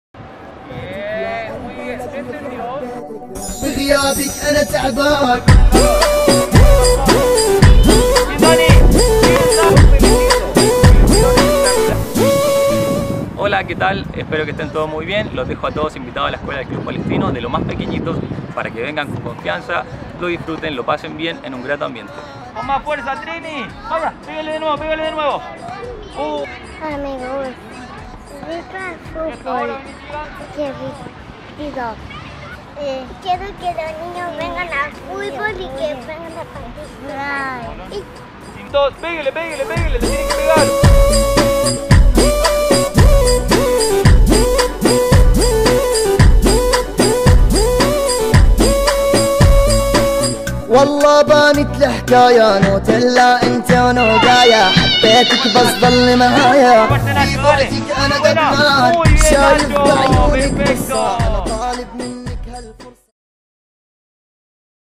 Escuela de Fútbol
Te invitamos a participar en la escuela de fútbol del Club Palestinos. Para niños de todas las edades. Contamos con todas las medidas para entrenar seguros y cuidarnos entre todos.